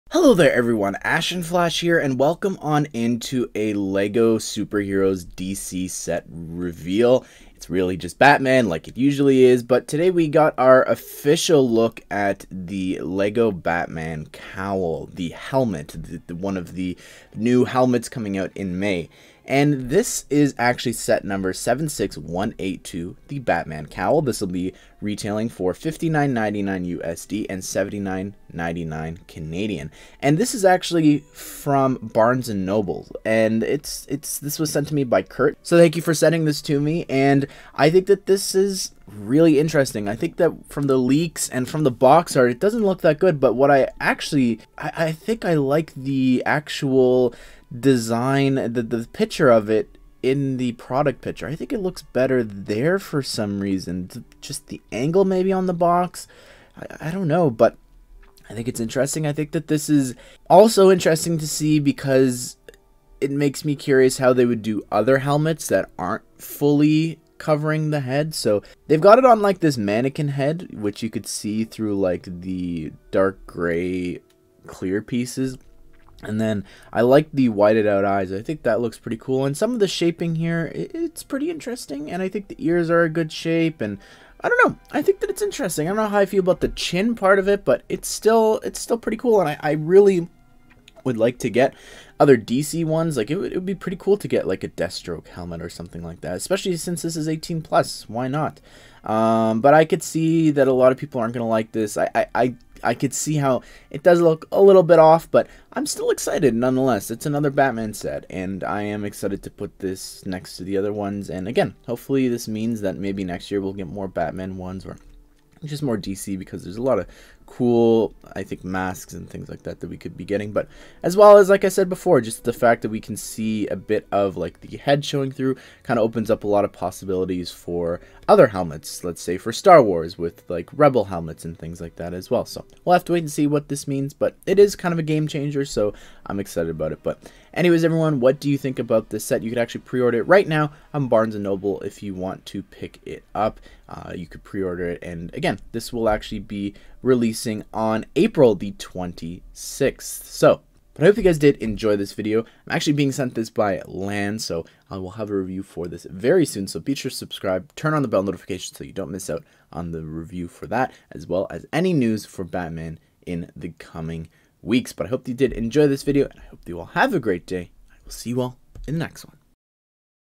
LEGO Batman Cowl 18+ Set Reveal
Today, Barns & Noble have revealed the new LEGO Batman Helmet available for pre-order right now and releasing on April 26th!